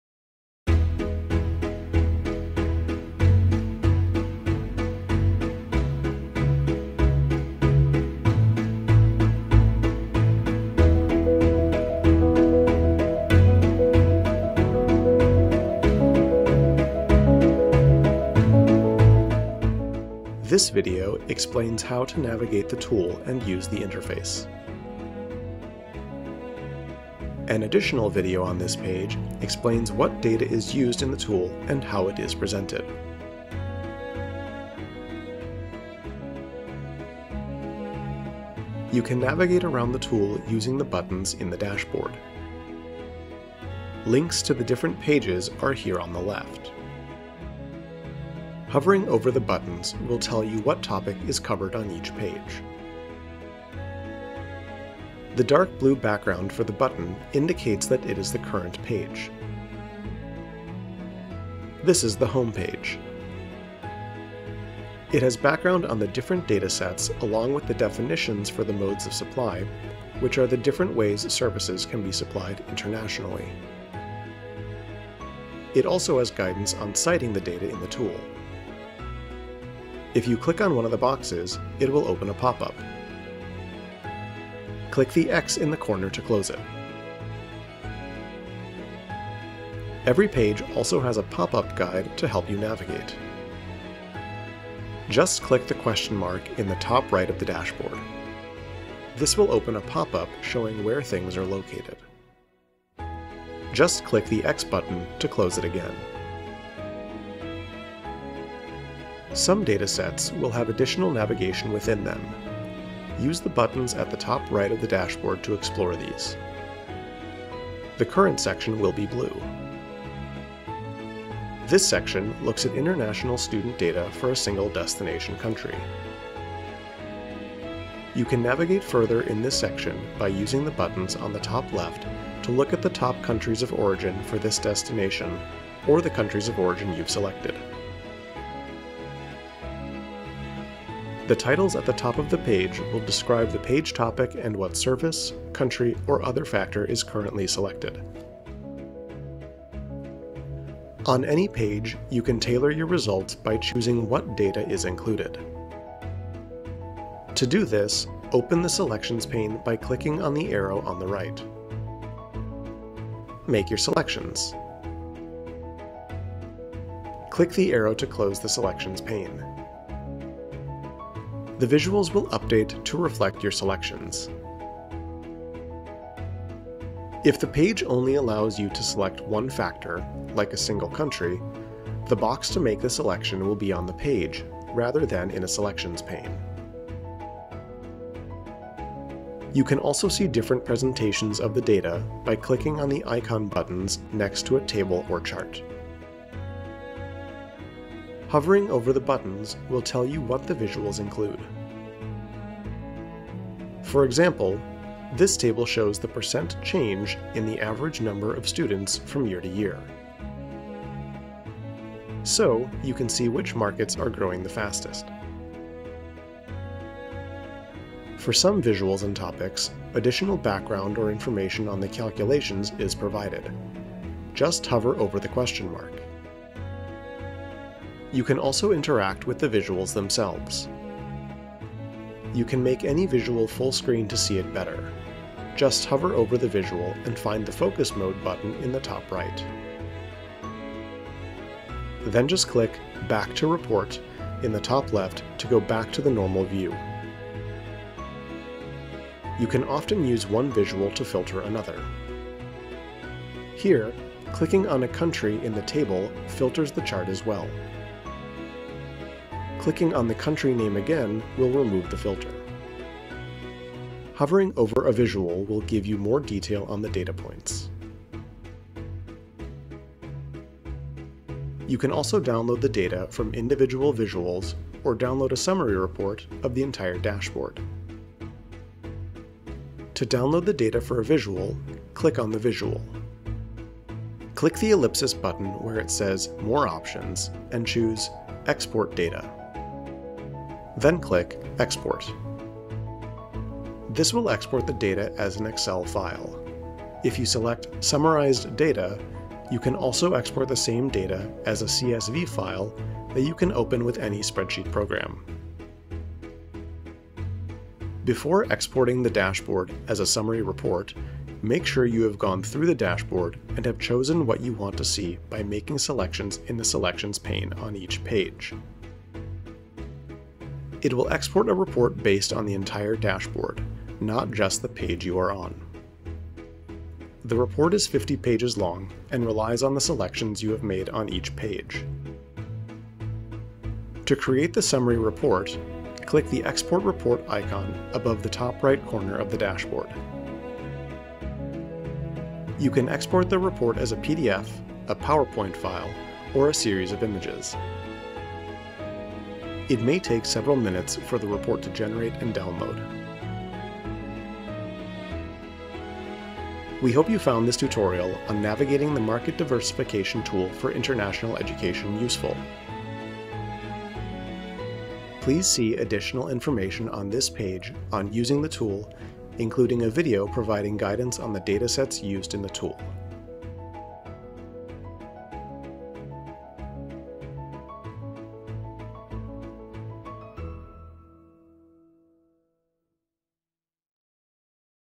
Market Diversification Tool for International Education: Navigating the Tool
The Market Diversification Tool for International Education can help you explore and compare potential markets for exporting education services. This video explains how to navigate the tool and use the interface.

00:00 Introduction
00:38 Moving Around in the Tool
02:30 Selecting Filters and Visuals
04:30 Exporting the Data and Summary Report
05:54 Conclusion